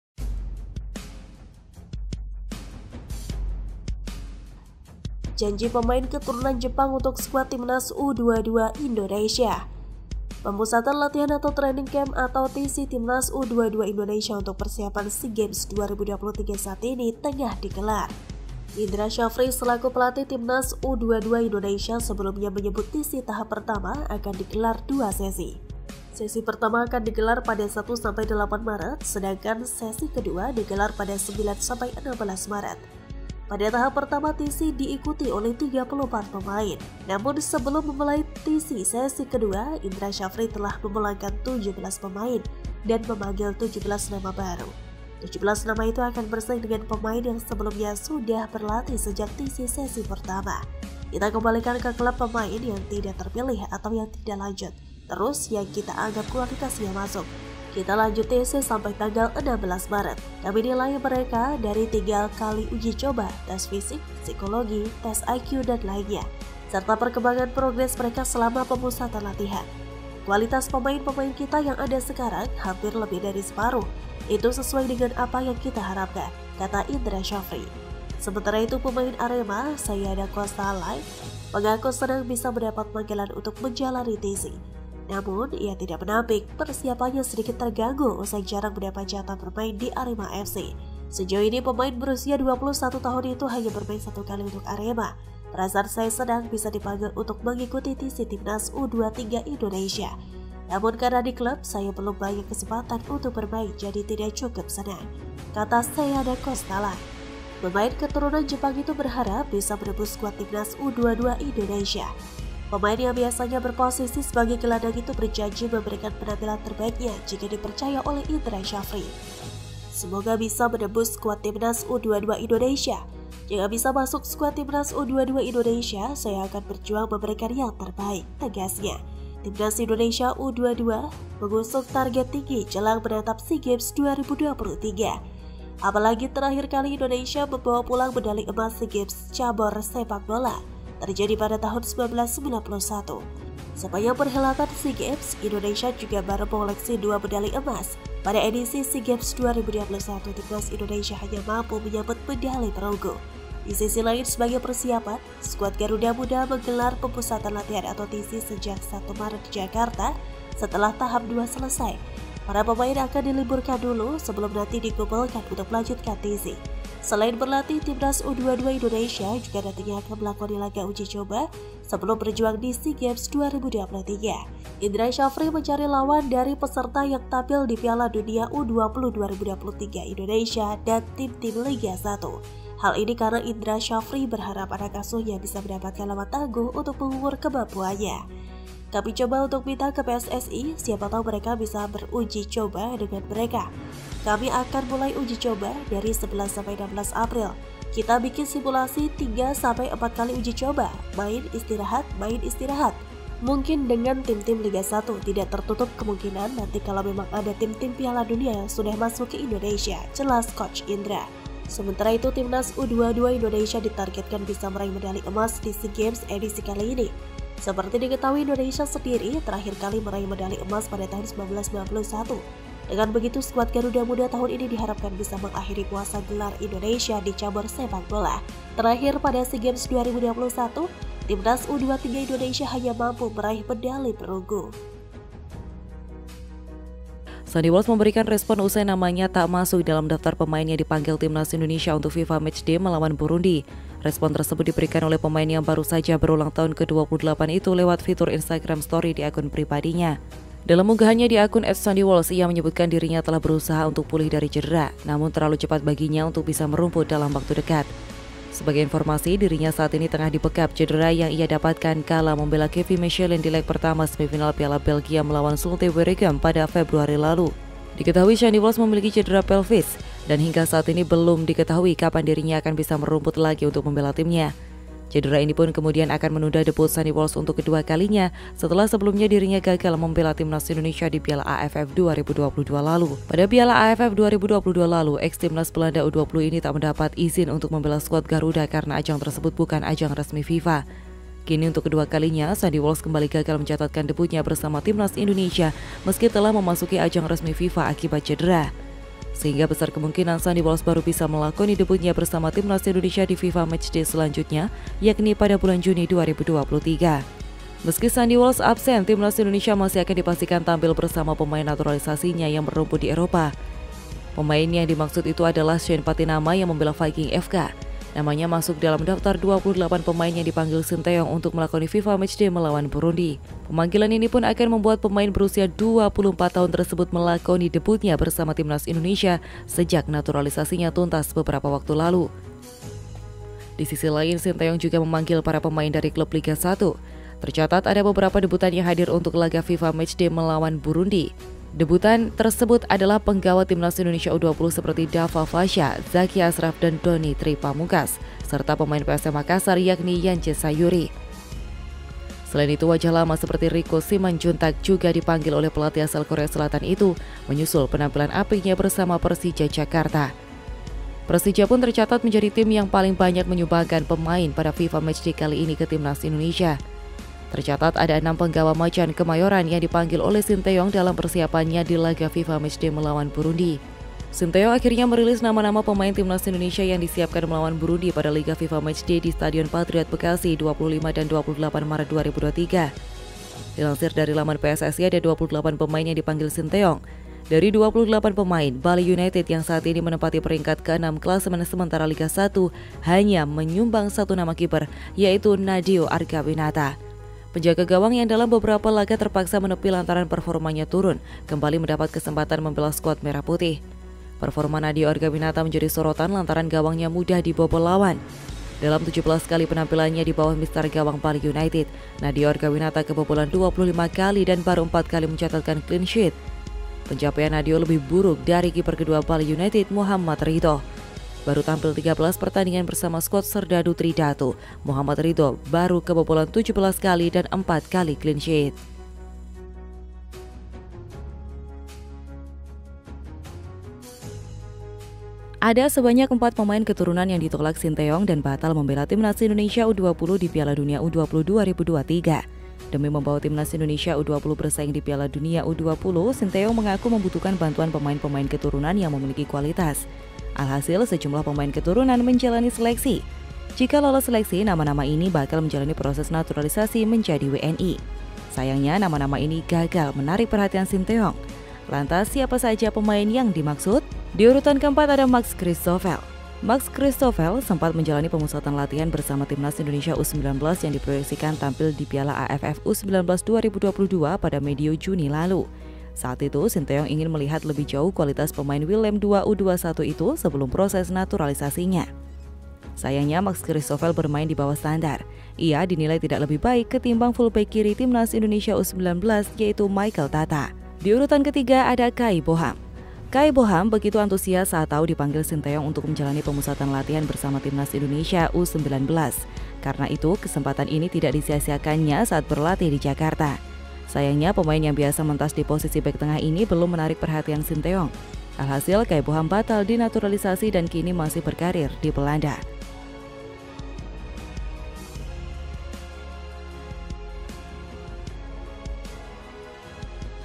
Pemain Keturunan Baru🔥 Seiya Da Costa Jadi Senjata Rahasia Coach Indra Sjafri Hadapi Sea Games 2023🔥
Janji Pemain Keturunan Jepang untuk Skuad Timnas U-22 Indonesia
Pemusatan latihan (training camp/TC) timnas U-22 Indonesia untuk persiapan SEA Games 2023 saat ini tengah digelar. Indra Sjafri selaku pelatih timnas U-22 Indonesia sebelumnya menyebut TC tahap pertama akan digelar dua sesi.
Sesi pertama akan digelar pada 1-8 Maret, sedangkan sesi kedua digelar pada 9-16 Maret. Pada tahap pertama, TC diikuti oleh 34 pemain.
Namun sebelum memulai TC sesi kedua, Indra Sjafri telah memulangkan 17 pemain, dan memanggil 17 nama baru. 17 nama itu akan bersaing dengan pemain yang sebelumnya sudah berlatih sejak TC sesi pertama.
"Kita kembalikan ke klub pemain yang tidak terpilih, atau yang tidak lanjut, Terus, yang kita anggap kualitasnya masuk, kita lanjut TC sampai tanggal 16 Maret. Kami nilai mereka dari tiga kali uji coba, test fisik, psikologi, test IQ, dan lainnya. Serta perkembangan progres mereka selama pemusatan latihan. Kualitas pemain-pemain kita yang ada sekarang, hampir lebih dari separuh, itu sesuai dengan apa yang kita harapkan,” kata Indra Sjafri
Sementara itu, pemain Arema, Seiya Da Costa Lay mengaku senang bisa mendapat panggilan untuk menjalani TC. Namun ia tak menampik, persiapannya sedikit terganggu usai jarang mendapat jatah bermain di Arema FC. Sejauh ini, pemain berusia 21 tahun itu hanya bermain satu kali untuk Arema.
"Perasaan saya senang bisa dipanggil untuk mengikuti TC timnas U-23 Indonesia, Namun karena di klub, saya belum banyak kesempatan untuk bermain jadi tidak cukup senang." jata Seiya Da Costa Lay